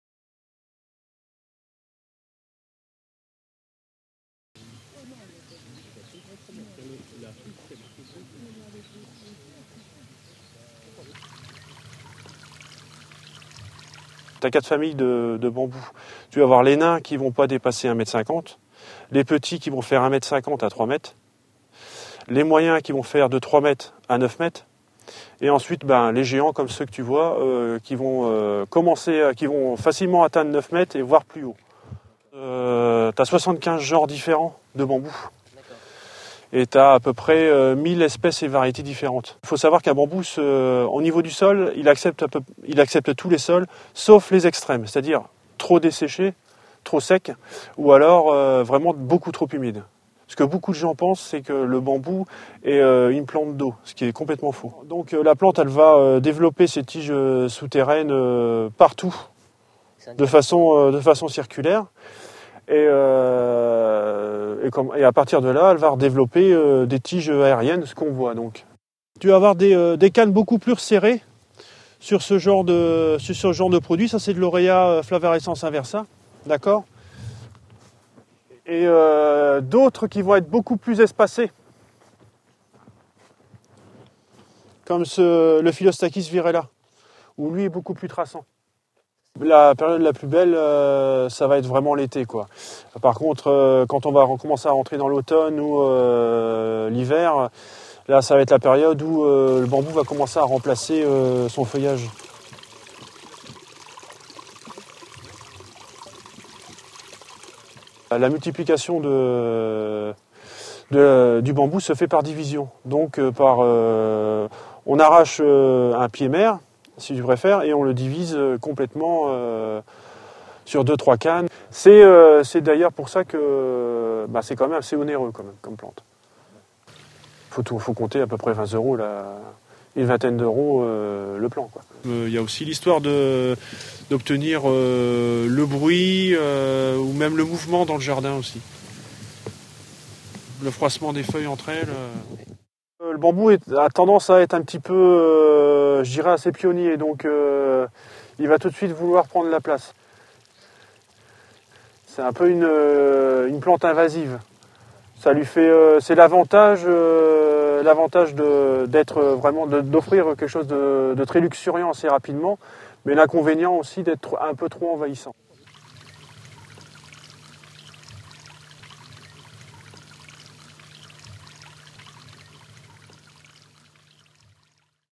Les BAMBOUS, des NAINS, des HAUTS, des GÉANTS pour tous les GOÛTS - CULTURE et ENTRETIEN
Eric SINGIER de la pépinière Jean HUCHET vous présente les bambous. Il y a quatre familles de bambous, les nains ne dépassant pas 1m50, les petits jusqu'à 3m, les moyens jusqu'à 9m et les géants qui dépassent les 9m. Il y a 75 genres de bambous et environ 1000 espèces et variétés différentes. Le bambou est de culture assez facile mais n'apprécie guère les extrèmes.